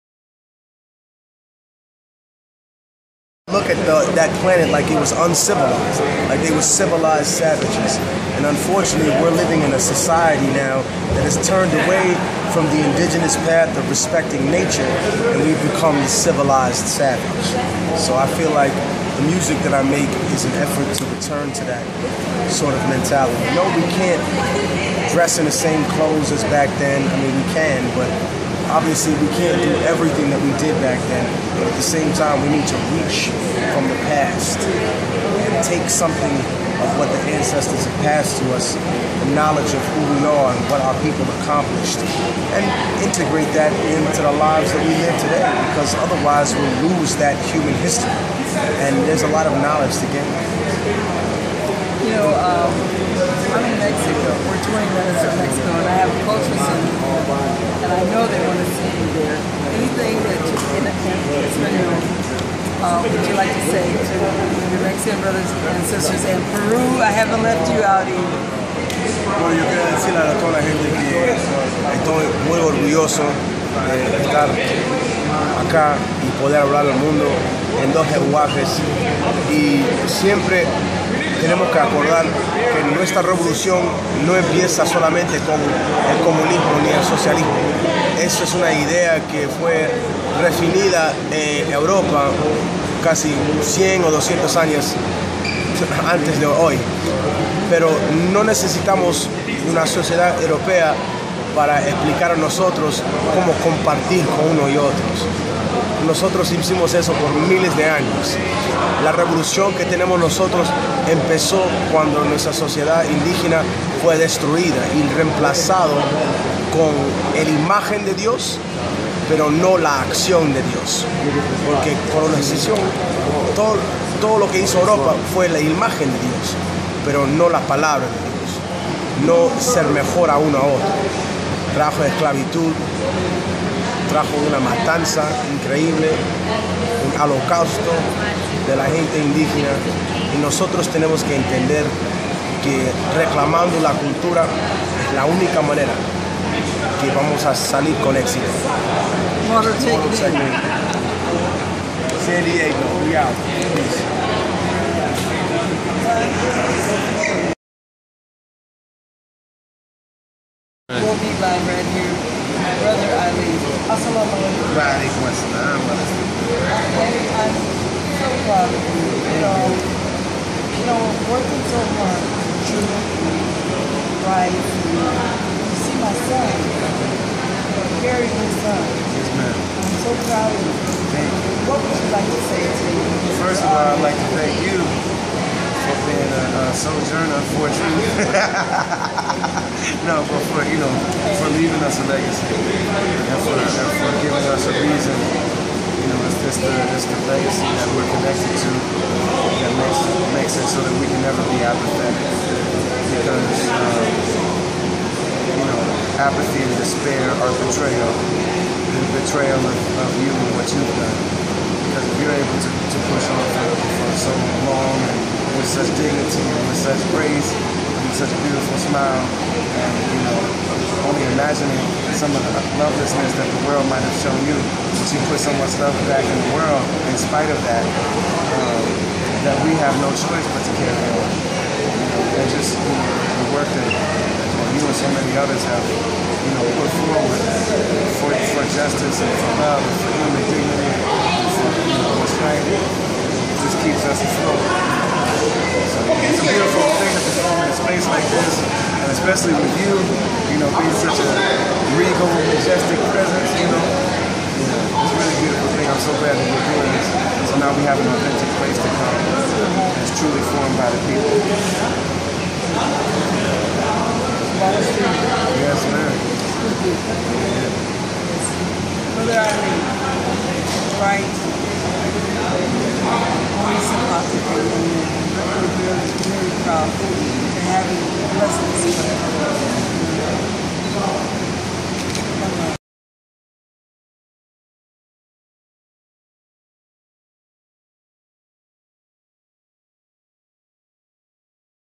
Immortal Technique and Brother Ali interview
Immortal Technique and Brother Ali interview with Makeda Dread at WorldBeat Center